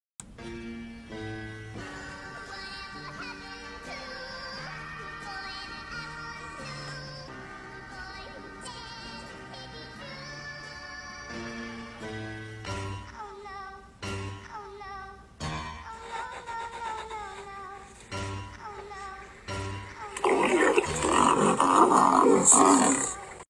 Lion Wakes Up Lioness 🦁
Lion sneaks up on Lioness 🦁